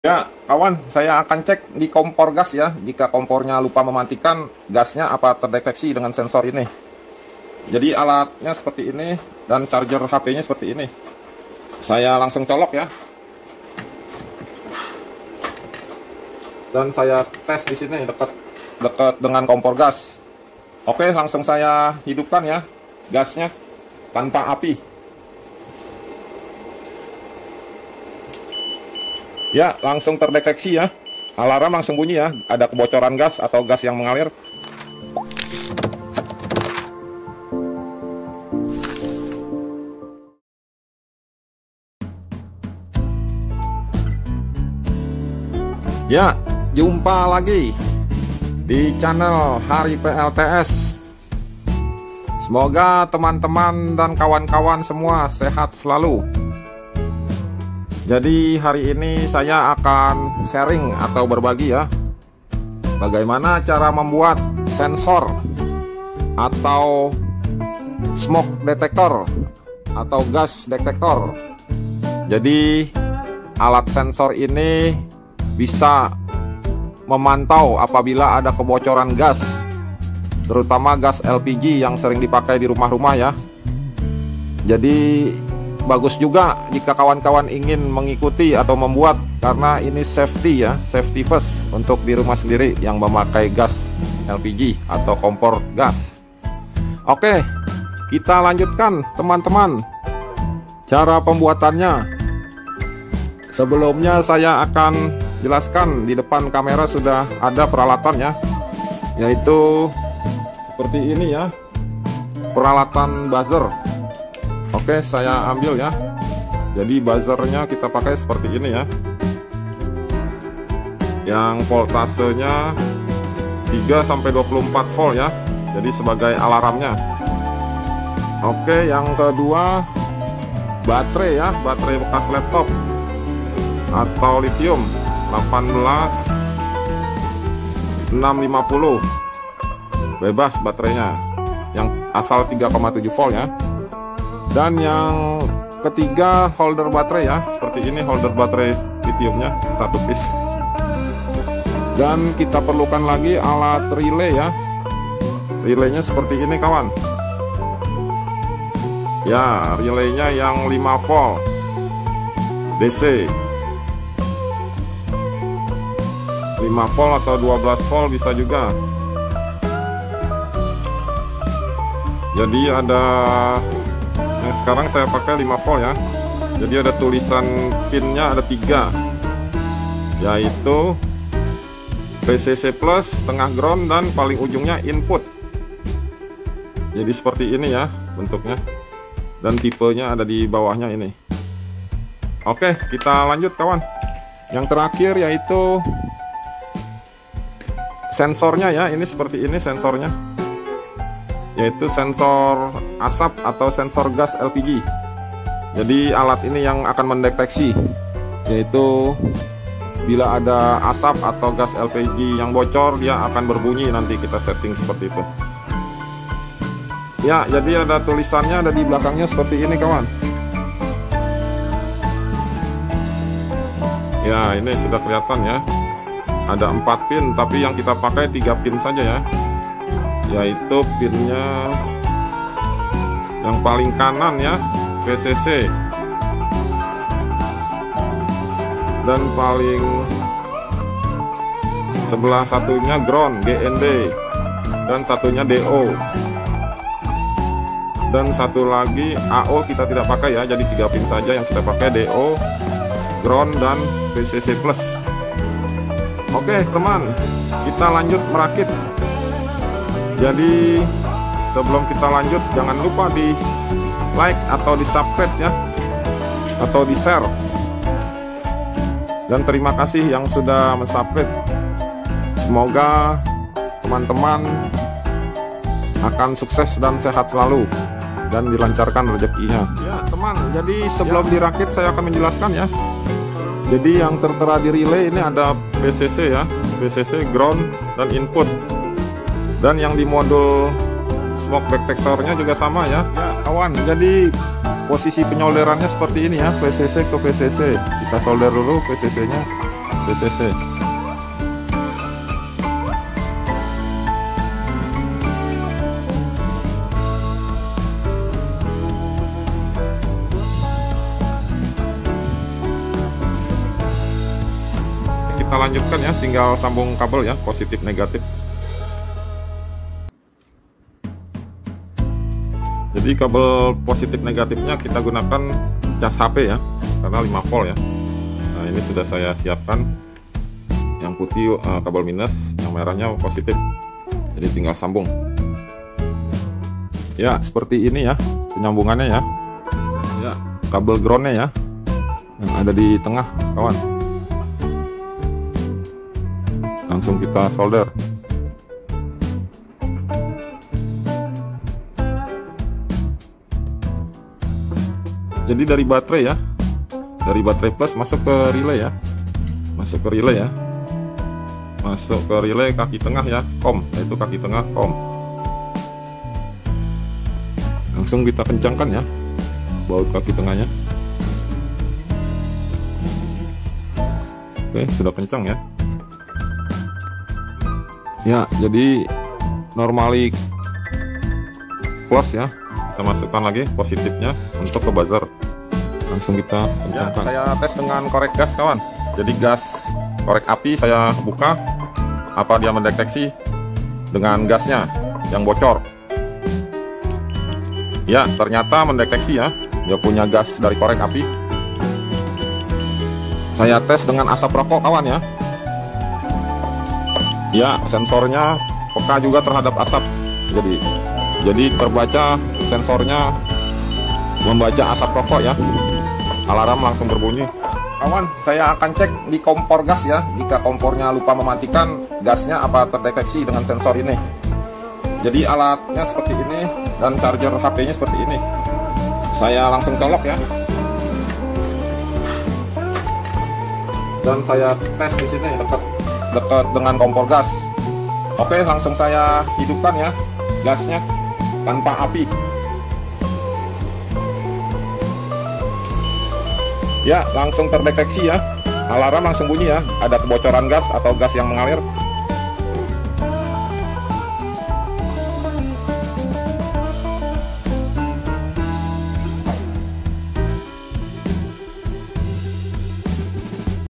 Cara Mudah Membuat Alarm Pendeteksi ASAP dan Kebocoran GAS ELPIJI Tanpa ARDUINO
Cara Mudah Membuat Alarm Pendeteksi Kebocoran GAS ELPIJI  Tanpa  ARDUINO
Alat - alat yang dibutuhkan.
1. Modul Relay 5 volt
2. SENSOR ASAP DAN DETEKSI GAS LPG
3. Battery bekas laptop 3.7 volt
4. Kabel secukupnya

Smoga bermanfaat.